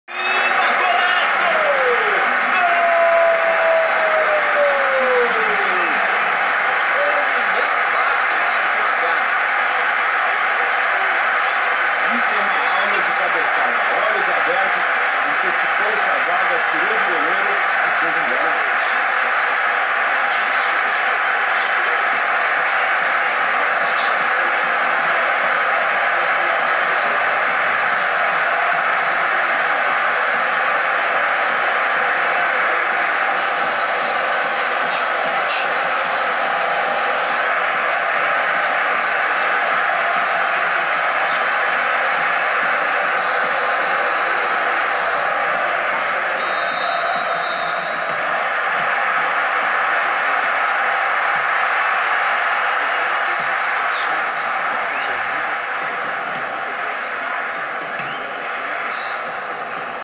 🇧🇪 Dries Mertens ⚽
🇧🇪 Dries Mertens é um futebolista Belga que atua como atacante. Atualmente joga no Napoli. Destaca-se pela velocidade, passes precisos e perigosas cobranças de falta.